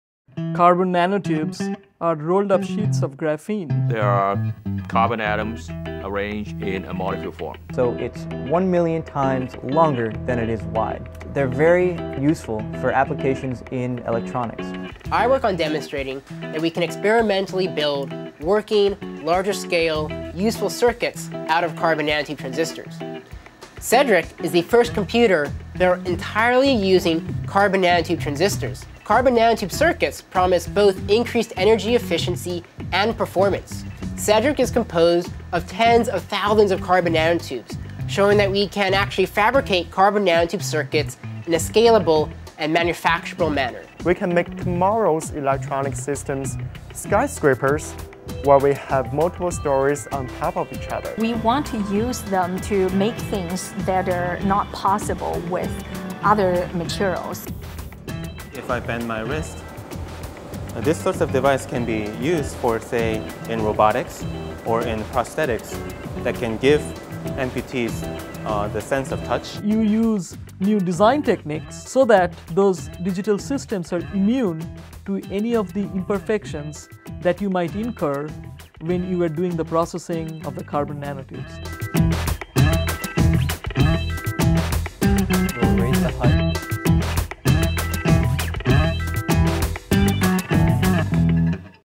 Stanford engineers build computer with carbon nanotubes
A team of Stanford engineers has built a basic computer using carbon nanotubes, a semiconductor material that has the potential to launch a new generation of electronic devices that run faster, while using less energy, than those made from silicon chips.